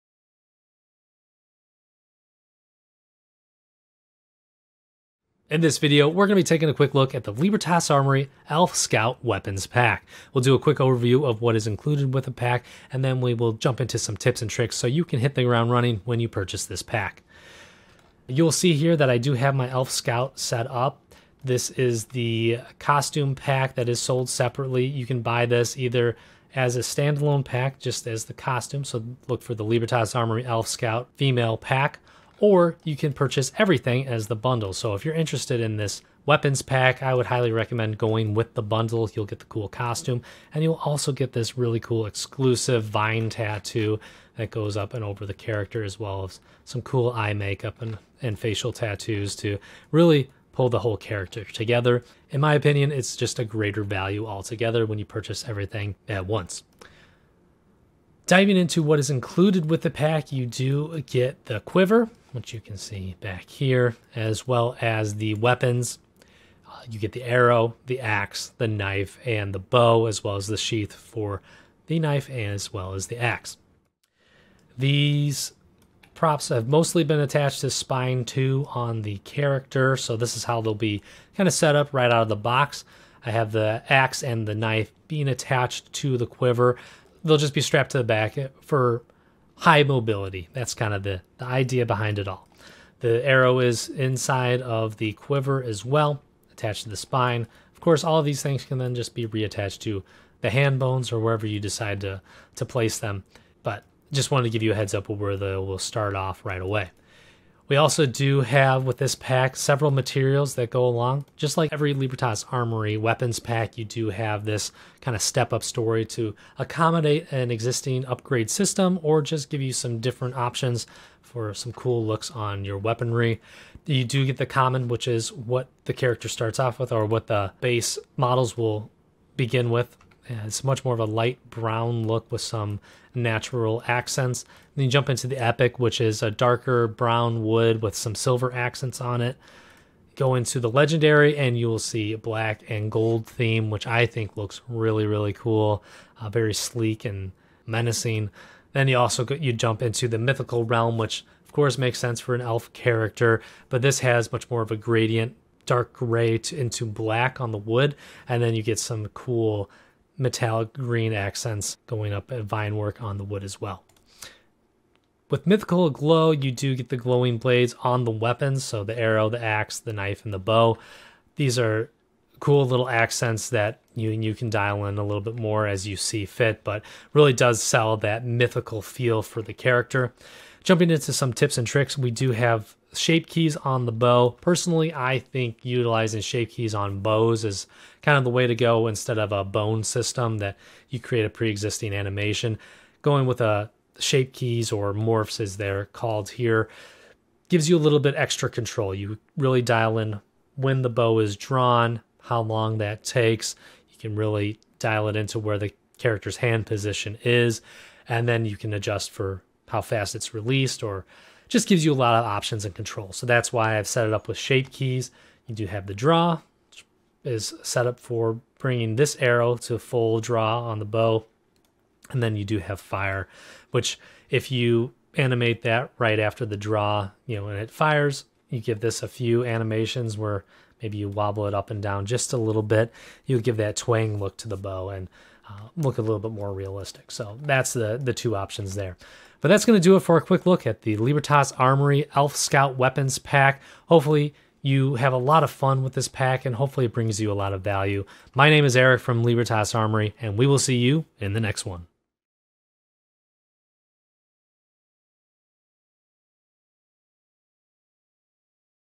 Elf Scout Weapons Pack - Overview
The Elf Scout Weapons Pack by Libertas Armory includes all the essential weapons your elven scouts need to keep their woodland forest safe as they investigate the dangers at the boarder of their realm.